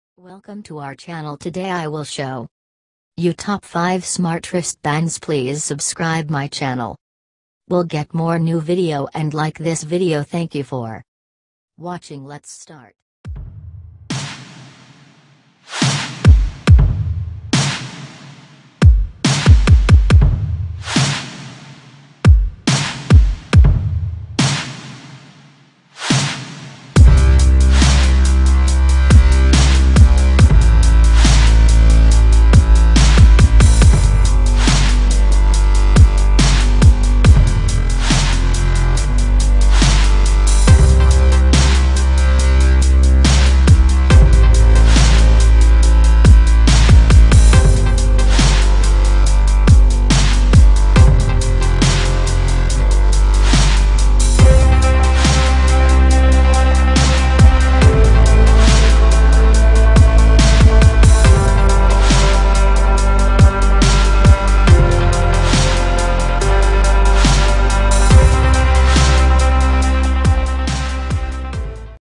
Top 5 best fitness tracker with heart rate monitor 2019
1--Bracelet Waterproof Heart Rate Monitor Health Fitness Tracker

3--Smart Bracelet Colors 3D dynamic Blood Pressure Heart Rate

MORVIDEO

Description:
Built-in 90mAh battery, working time about 7 days, standby time about 15 days
* IP68 waterproof and dustproof ( you can wear it to rain, wash hands and take a bath, but don't put it into hot water)
* Blood pressure and heart rate monitor
* Smart calls & messages reminder: Call, Twitter, Facebook, Wechat etc, all will be sent to wrist display and with vibration alert.
* A variety of sports patterns, APP operation view pedometer,calories,distance
* Support iOS 8.2 version above and Android 4.4 version above
* More function: Sleep monitor, Blood pressure / Heart rate monitor, Movement Monitor (steps,distance,calories), Pedometer, Message sync, Incoming call, Alarm clock,vibration
  Bracelet support language: Chinese (Simplified, Traditional), Japanese, English, French, German, Italian, Spanish, Russian, Portuguese, Polish, Malay
APP support language: Chinese (Simplified, Traditional), Japanese, English, French, German, Italian, Spanish, Russian, Portuguese, Korean, Malaysian